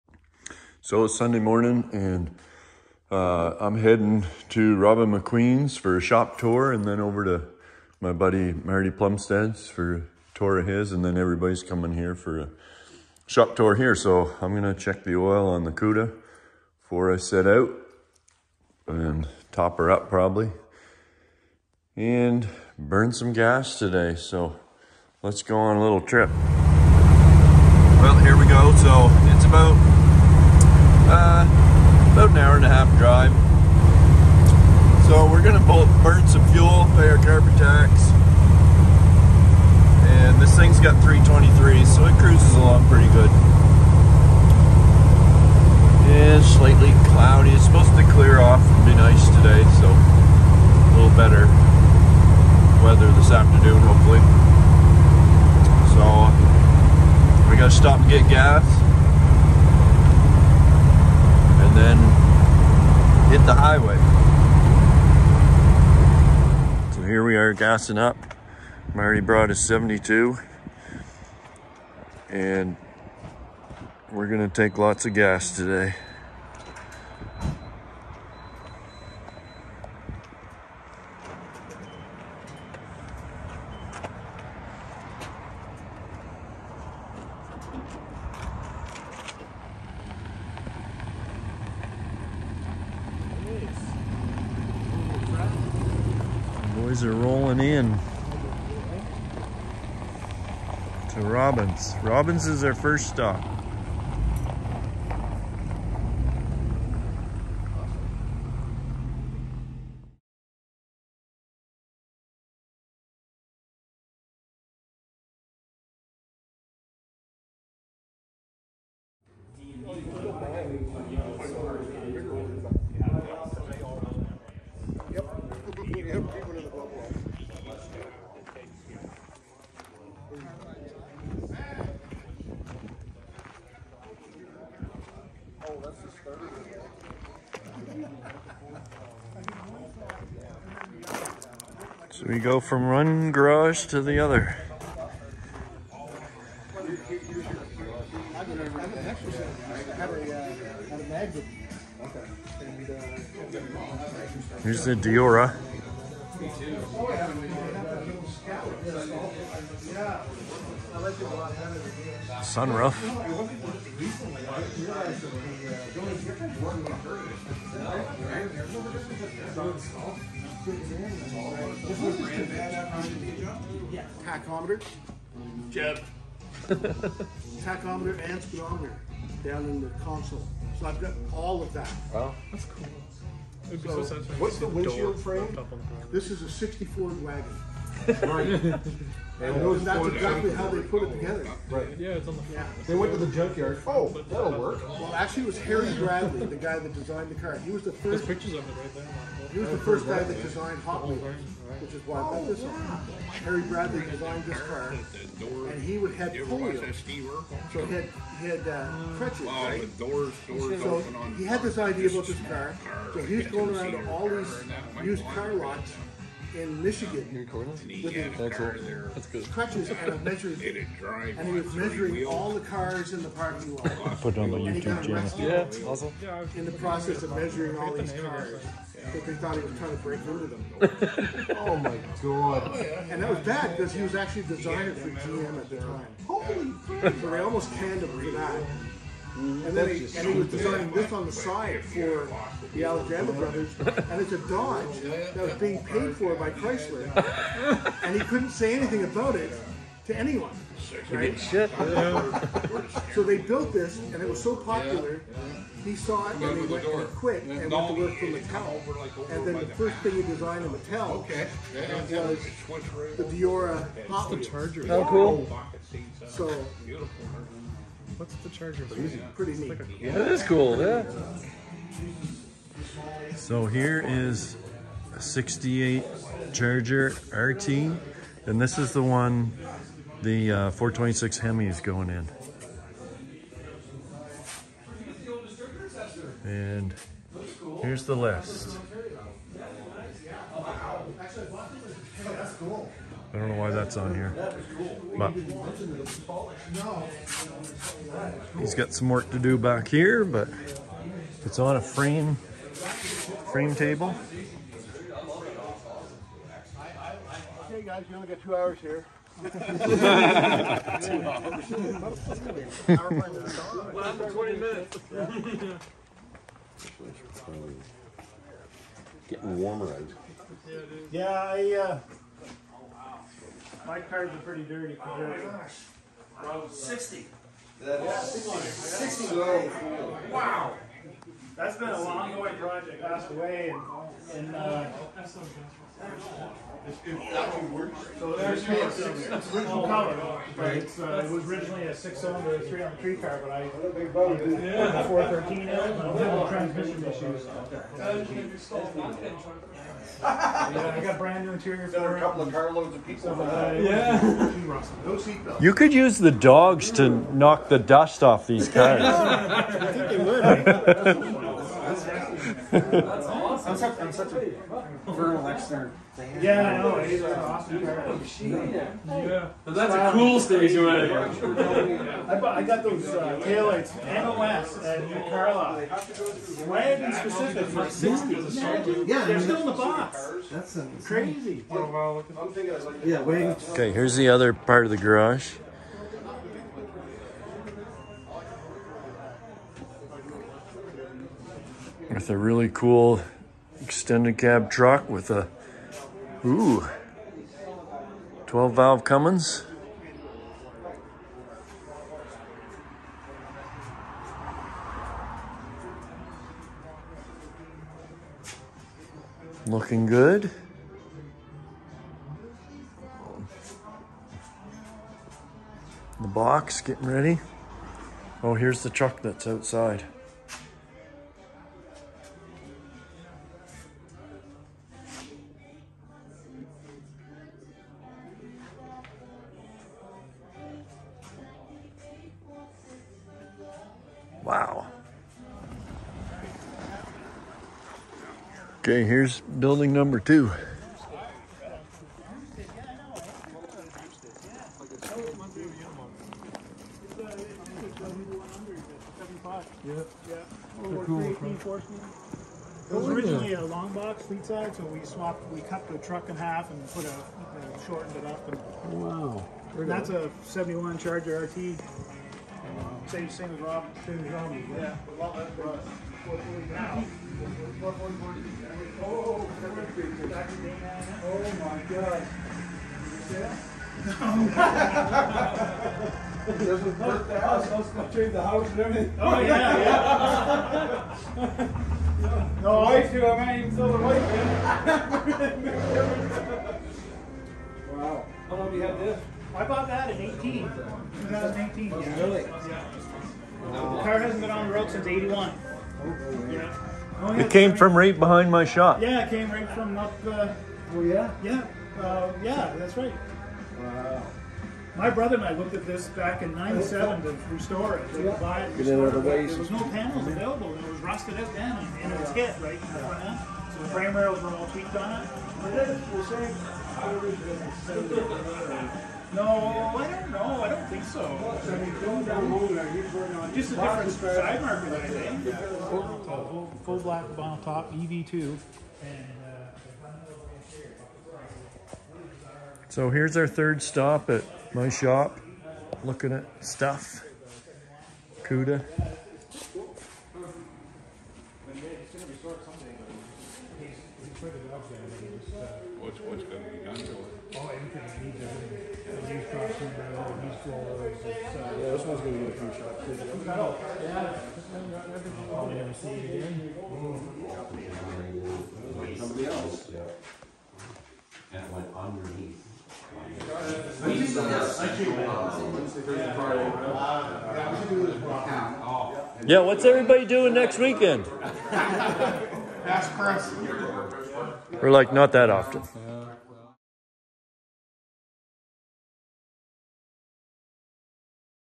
St. John guys coming for shop tours!
Our friends from St. John come up to tour Robin, Marty and my shops. Tag along to see some cool stuff!